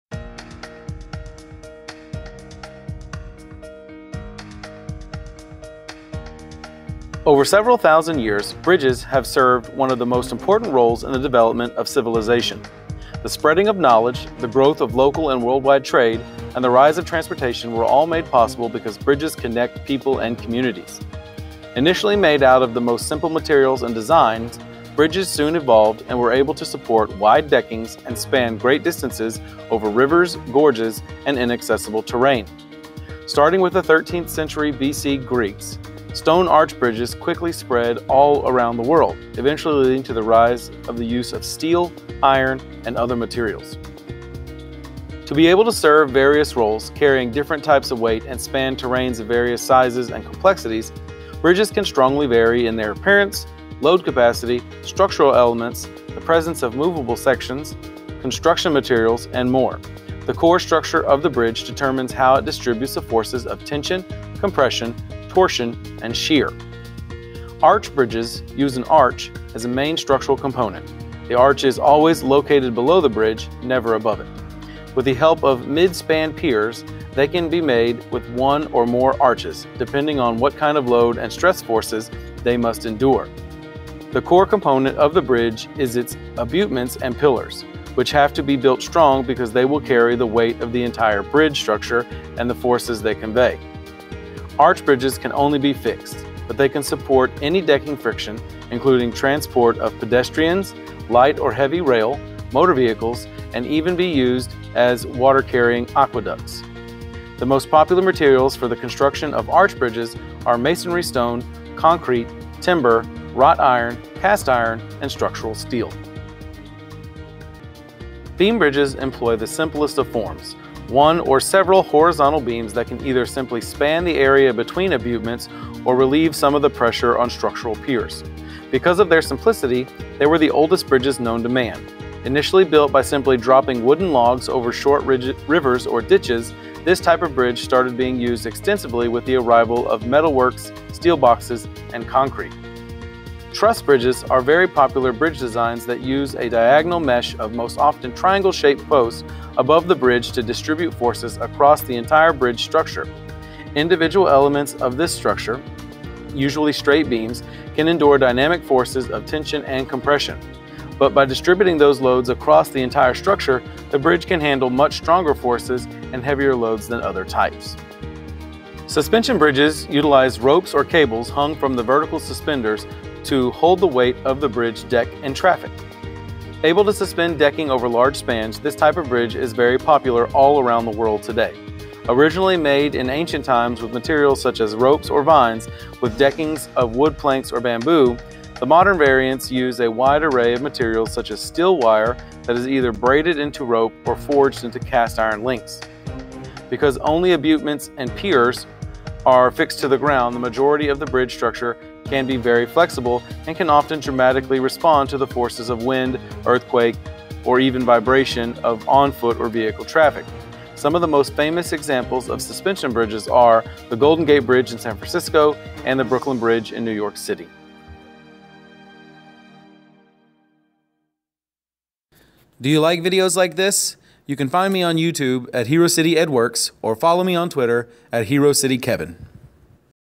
Engineering History: Bridges
This video explains the history and engineering principles behind 4 types of bridges. You can print the reading excerpt and word search that goes with the video